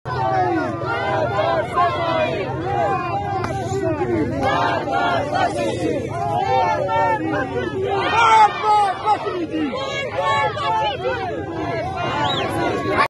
خاش مرگ بر بسیجی؛ شعاردهی مردم انقلابی به مناسبت سالگرد جمعه خونین زاهدان
خاش
مرگ بر بسیجی؛ شعاردهی مردم انقلابی به مناسبت سالگرد جمعه خونین زاهدان

جمعه ۷ مهر ۱۴۰۲